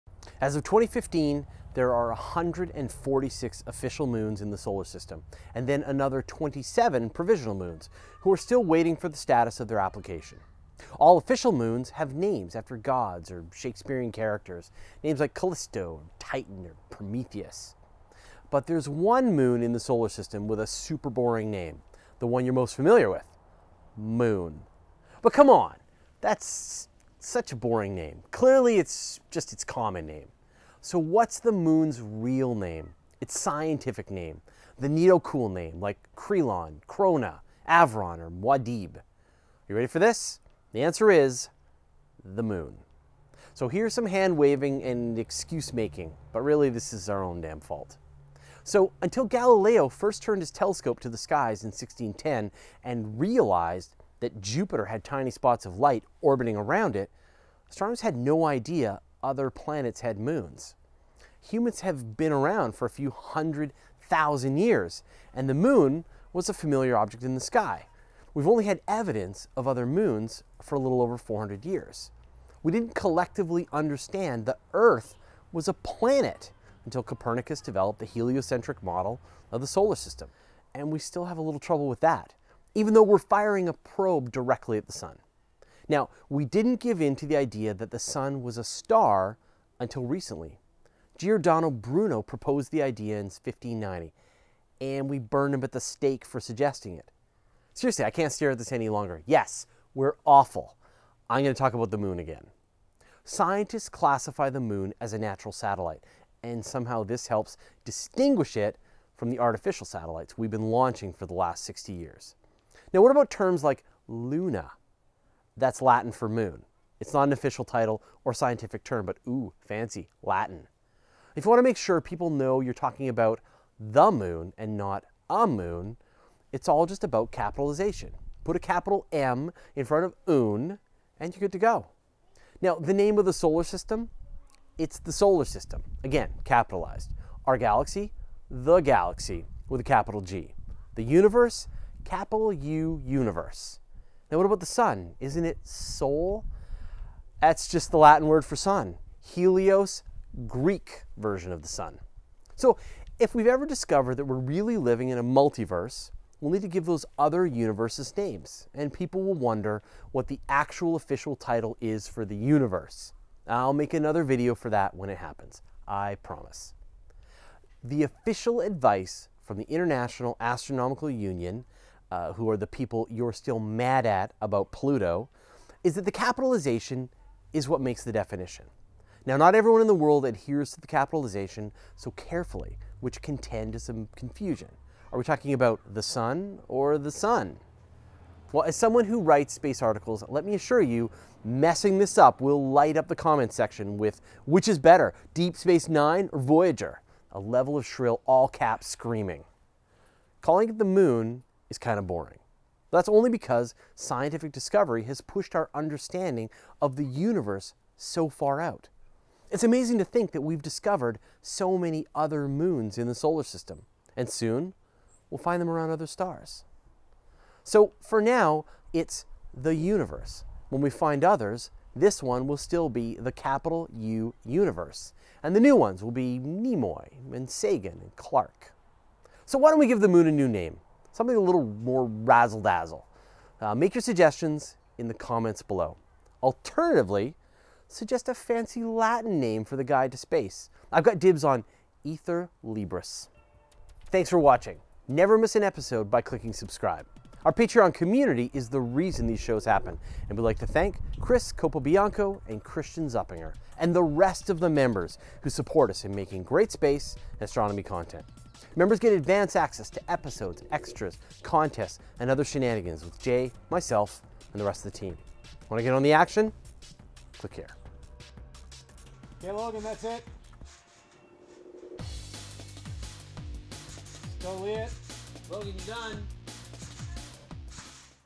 What Is The Moon's Real Name?
We call it the Moon, but... what's its real name? You know, the name that scientists call the Moon.

Edited by: Chad Weber

As of 2015, there are 146 official moons in the Solar System, and then another 27 provisional moons, who are still waiting on the status of their application. All official moons have names after gods or Shakespeare characters. Names like Callisto, Titan, or Prometheus. But there’s one moon in the Solar System with a super boring name… the one you’re most familiar with: Moon.

But come on, that’s such a boring name. Clearly that’s just its common name. So what’s the Moon’s real name? Its scientific name. The neato cool name. Like Krelon, Krona, Avron or Mua’Dib.

Are you ready for this? The answer is: The Moon. Here’s some hand-waving and excuse making. Really, this is our own damn fault. Until Galileo first turned his telescope to the skies in 1610, and realized that Jupiter had tiny spots of light orbiting around it, astronomers had no idea other planets had moons.

Humans have been around for a few hundred thousand years, and the Moon was a familiar object in the sky. We’ve only had evidence of other moons for a little over 400 years. We didn't collectively understand the Earth was a planet until Copernicus developed the heliocentric model of the Solar System.

We still have a little trouble with that, even though we’re firing a probe directly at the Sun. We didn't give into the idea that the Sun was a star until recently. Giordano Bruno proposed the idea in 1590 and we burned him at the stake for suggesting it. Seriously, I can’t stare at this any longer. Yes, we’re awful. I’m going to talk about “the Moon” again.

Scientists classify the Moon as a natural satellite. Somehow this helps distinguish it from the artificial satellites we've been launching for the last 60 years.

What about terms like “Luna”? That’s Latin for Moon. It’s not an official title or scientific term, but ooh, fancy. Latin.

If you want to make sure people know you’re talking about “The Moon” and not “a moon”, it’s all about capitalization. Put a capital “M” in front of “oon” and you’re good to go.

The name of our solar system? It’s the Solar System (again, capitalized). Our galaxy? The Galaxy with a capital G. The universe? Capital U Universe.

What about the Sun? Isn't it “sol”? That’s just the Latin word for “sun”. Helios? Greek God version of the Sun.

If we ever discover that we’re really living in a multiverse, we’ll need to give those other universes names. And people will wonder what the actual official title is for the Universe. I’ll make another video when that happens, I promise.

The official advice from the International Astronomical Union, who are the people you’re still mad at about Pluto, is that the capitalization is what makes the definition.

Not everyone in the world adheres to the capitalization so carefully, which can tend to some confusion. Are we talking about the sun or the Sun? As someone who writes space articles, let me assure you, messing this up will light up the comments section with “Which is better Deep Space 9 vs. Voyager” level of shrill all caps screaming.

Calling it “the Moon” is kind of boring, but that’s only because scientific discovery has pushed our understanding of the Universe so far out. It’s amazing to think that we've discovered so many other moons in the Solar System, and soon, we’ll find them around other stars.

So, for now it’s The Universe. When we find others, this one will still be THE Capital-U Universe and the new ones will be Nimoy and Sagan and Clarke.

Why don’t we give the Moon a new name. Something with a little more razzle-dazzle. Make your suggestions in the comments below. Alternately, suggest a fancy Latin name of “Guide to Space”, I've got dibs on “Aether Libris”.